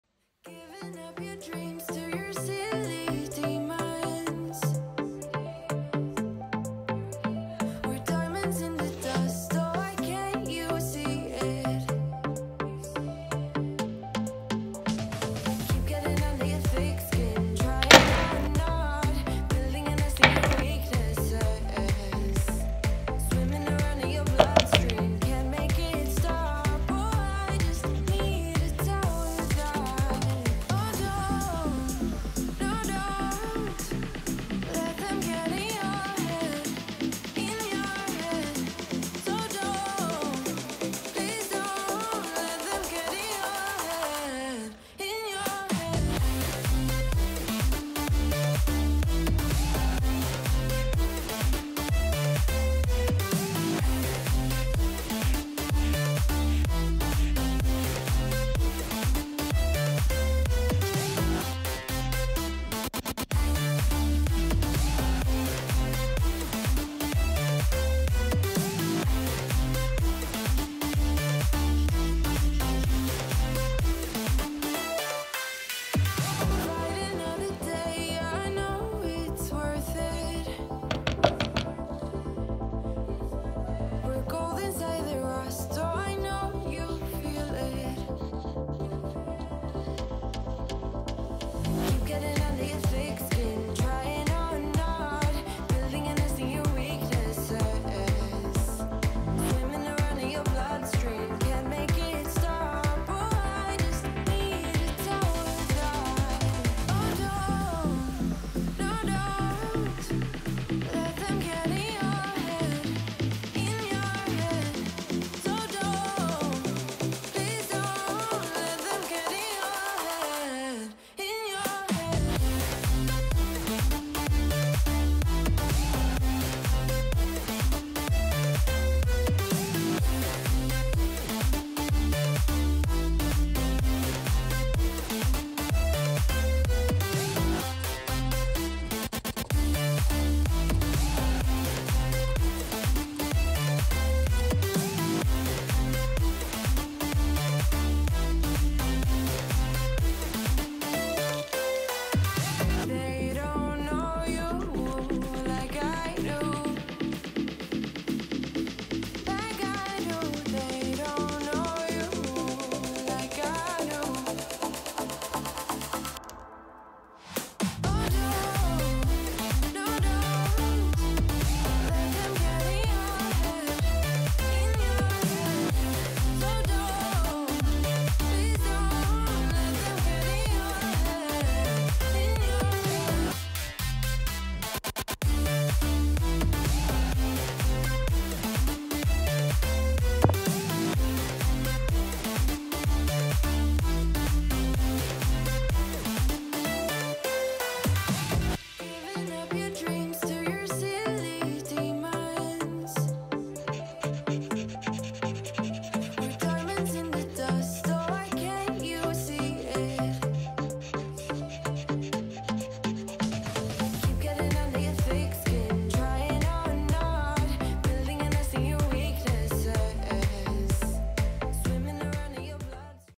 How to draw a Crying Bear with Posca Markers
Hello everyone, friends in this video, you and I will draw a Crying Bear , best pictures for you!
I am a beginner artist. I like to draw Posca  markers.
Go to the channel there are more videos for you.
Write in the comments that you want me to draw